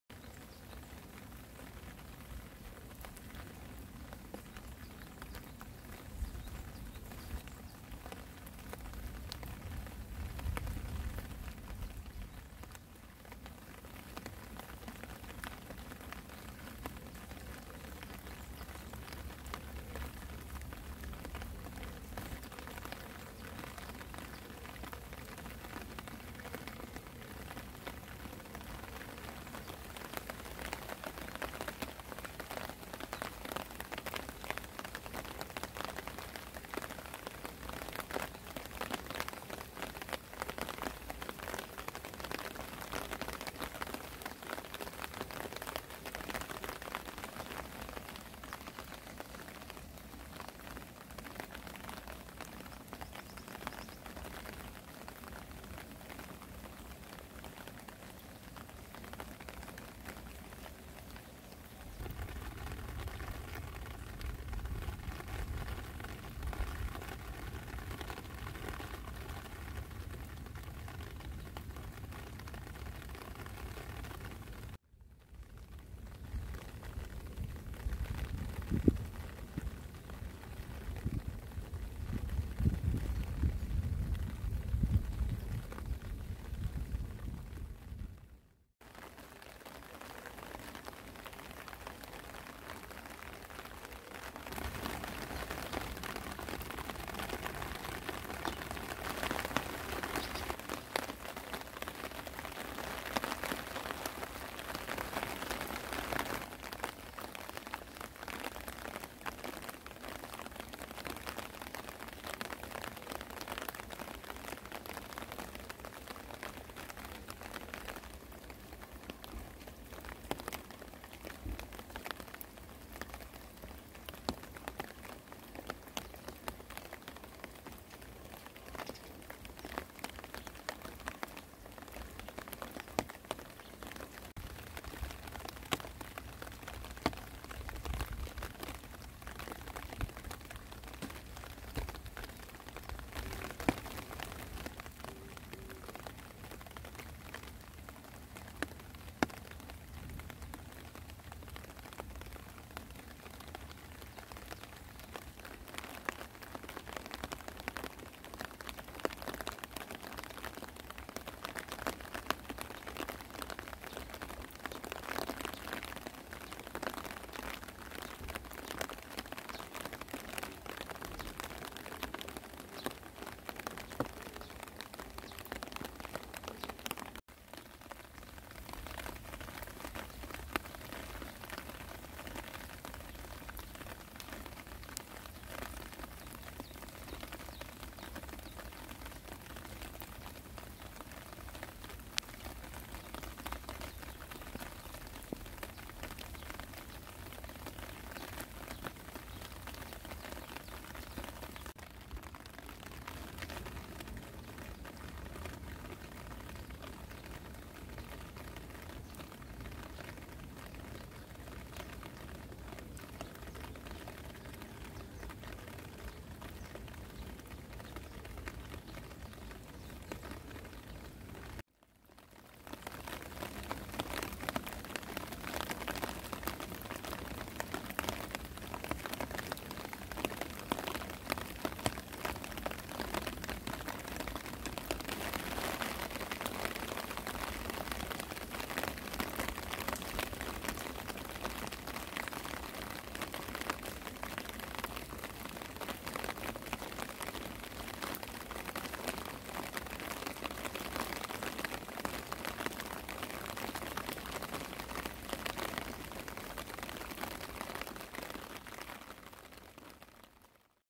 봄비 소리를 들어봅시다
2023년 4월 5일 봄비 내리는 날
학교 주변 관찰하기